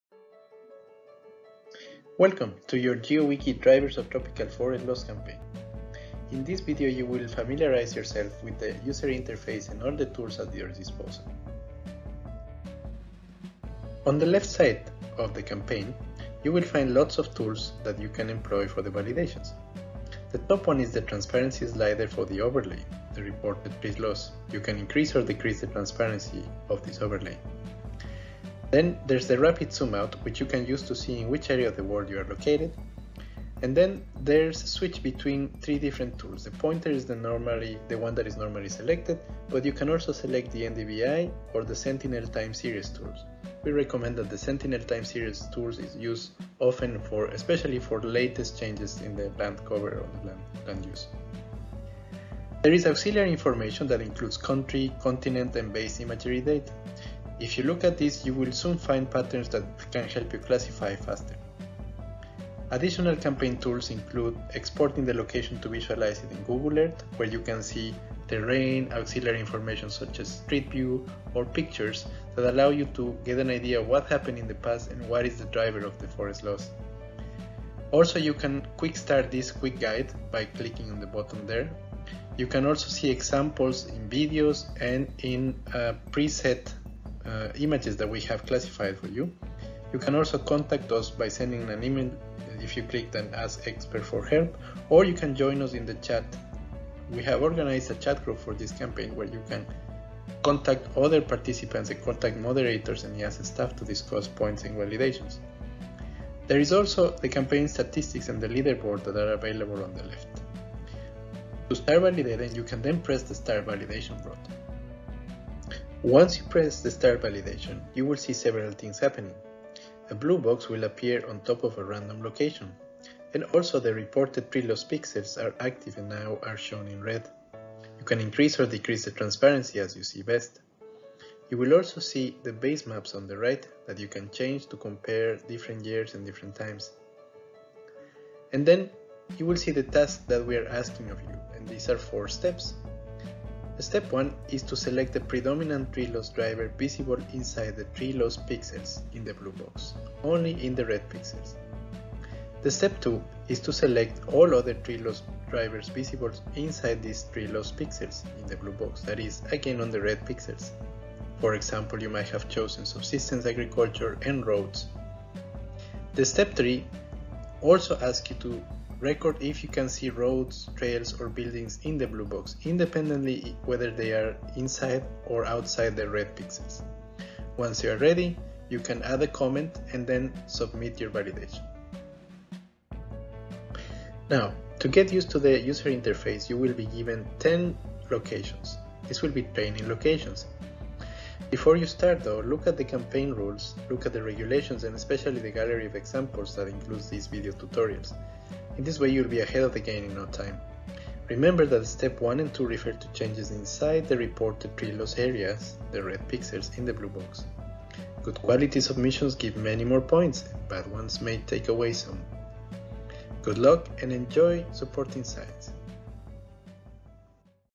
Quick start guide - Drivers of Tropical Forest Loss
Learn to use our Geo-Wiki interface for the Drivers of Tropical Forest Loss campaign and you'll be ahead of the game in no time!